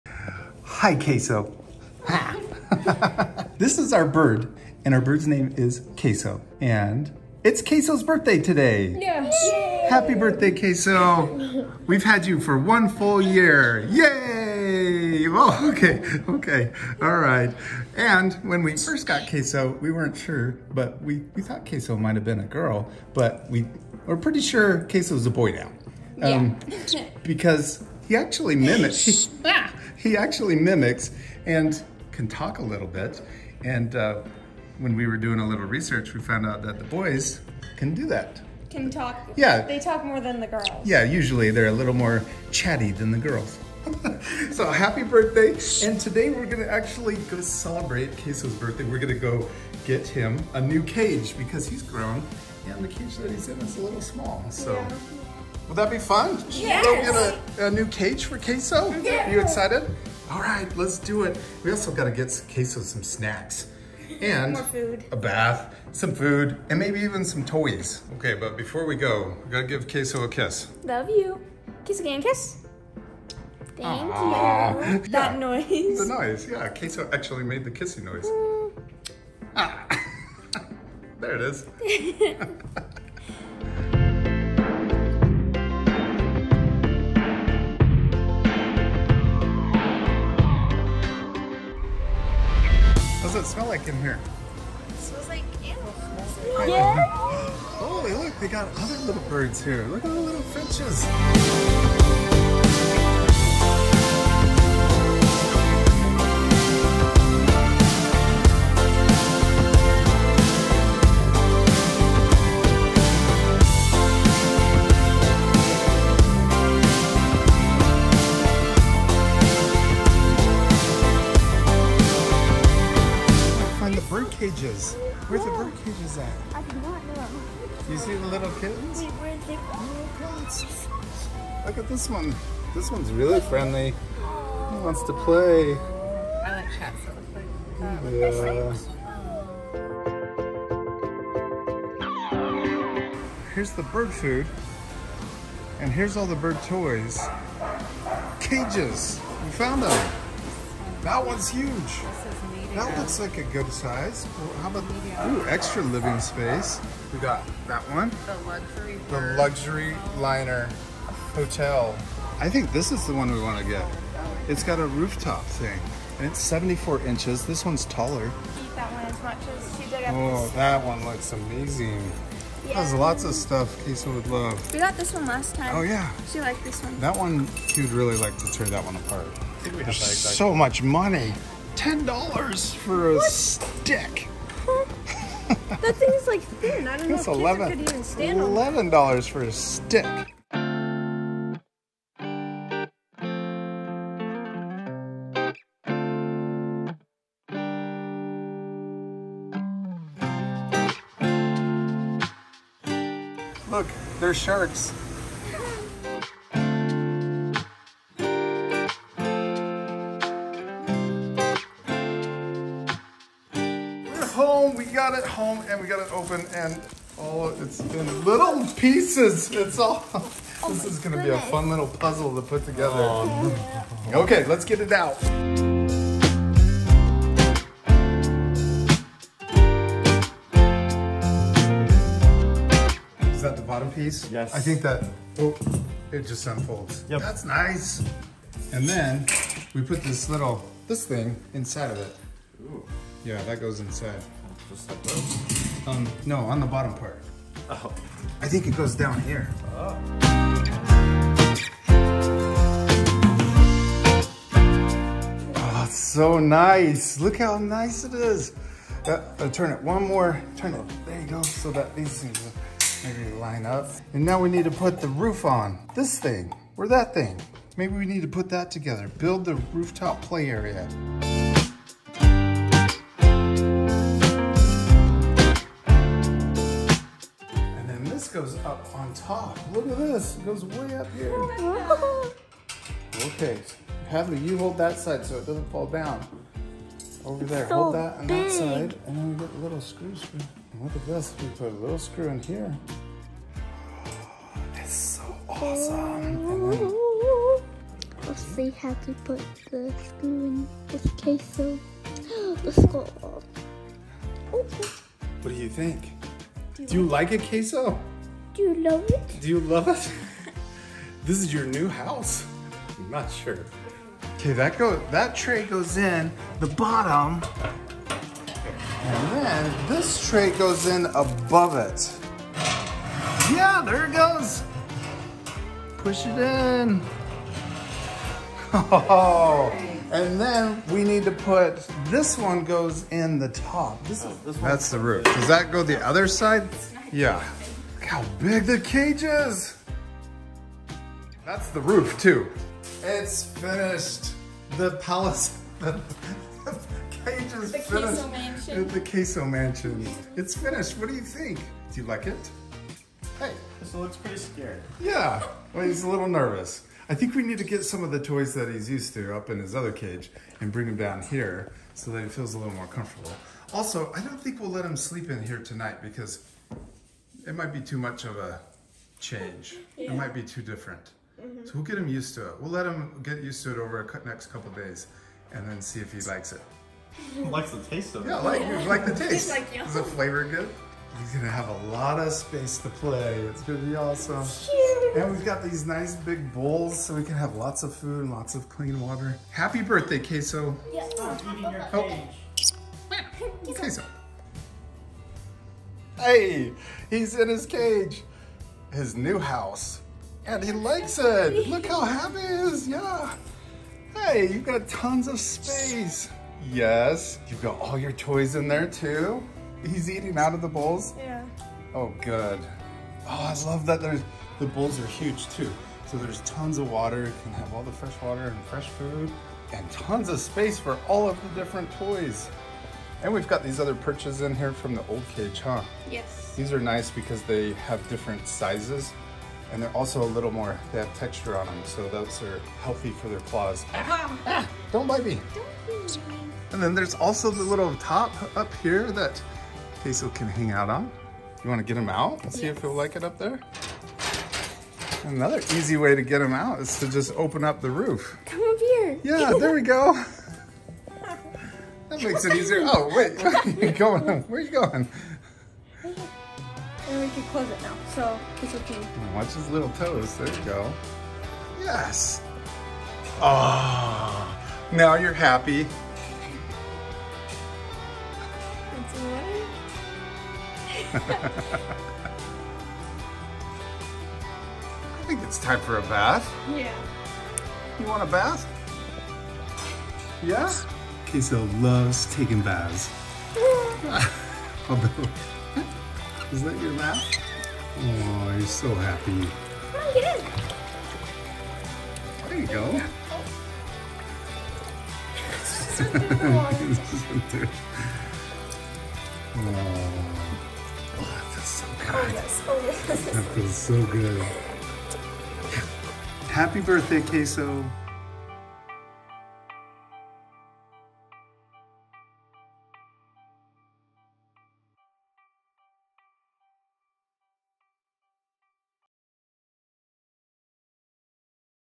Happy Birthday K-SO!
It's K-So's birthday! We've had him for one year, and he's grown so much! It's time for a new home.